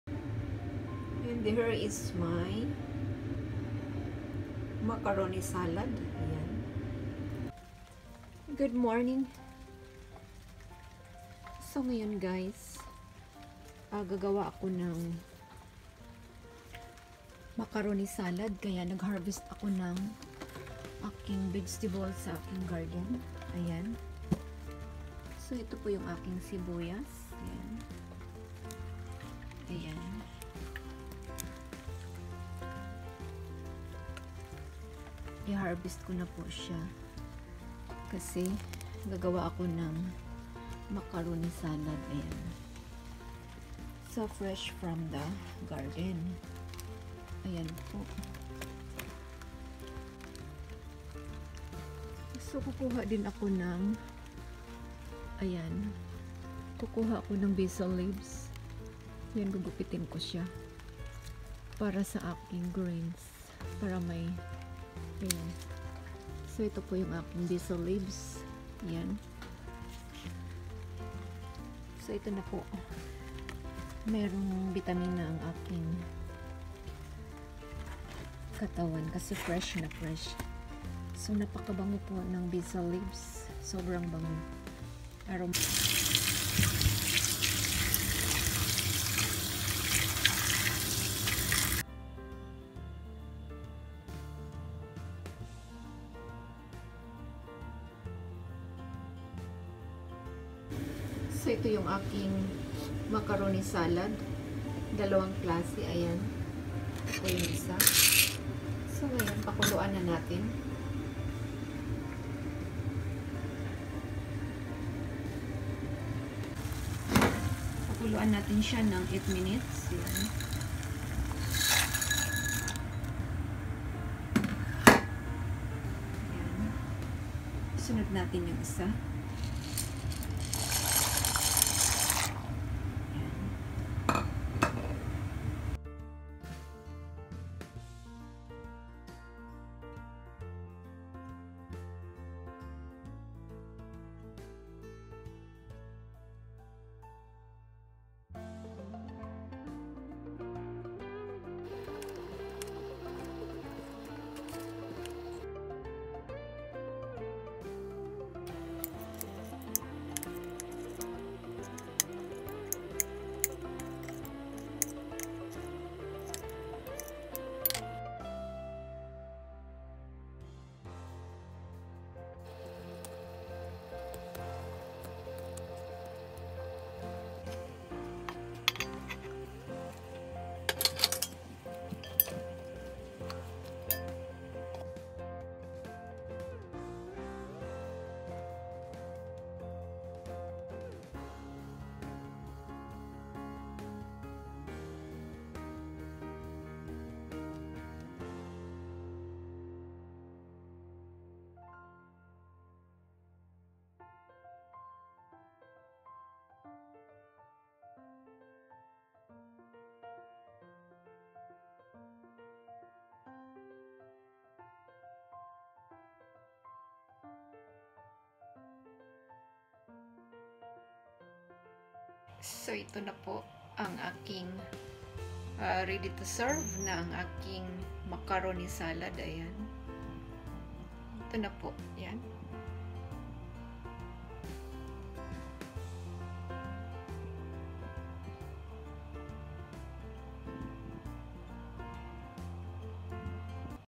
How To Make Macaroni Salad Cooking, Gardening,Traveling
Macaroni Salad:

Ingredients:

1 pack Macaroni
Chicken
Onion
Tomato
Chop carrot
Mayonnaise
Pineapple fruits Jam
Mild soy sauce to taste